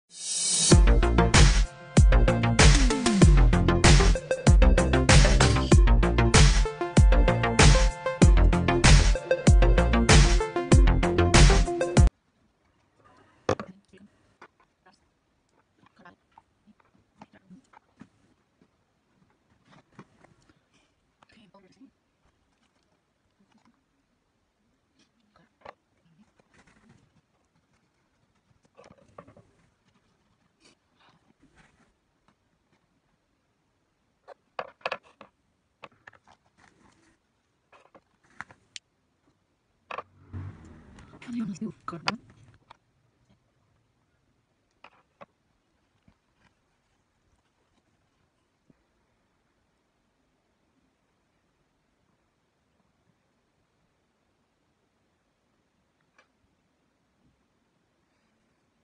Skeleton at Seema the make up studio
Ghost makeup by Seema gurung